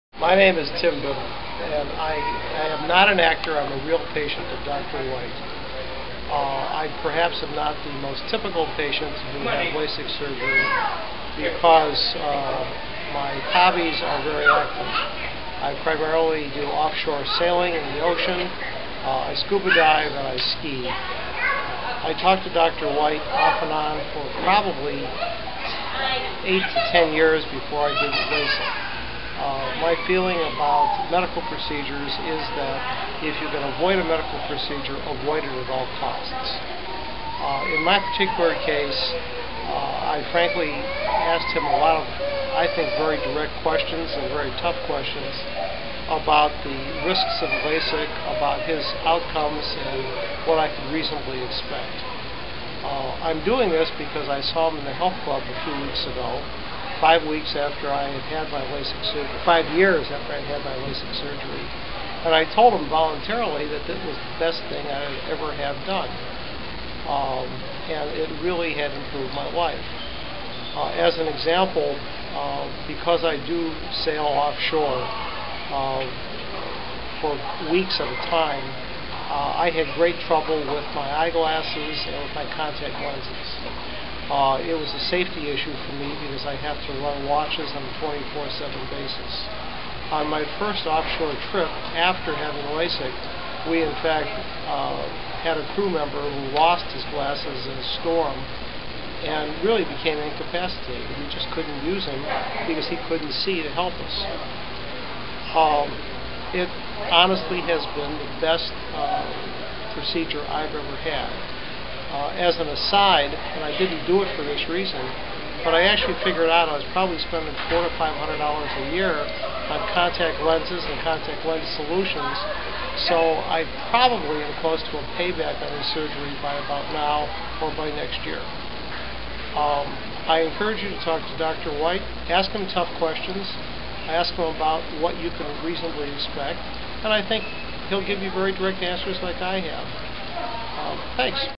Tim Bittel Talks about Skyvision LASIK
Skyvision Centers Patient Tim Bittel talks about his experience having LASIK done by Dr. White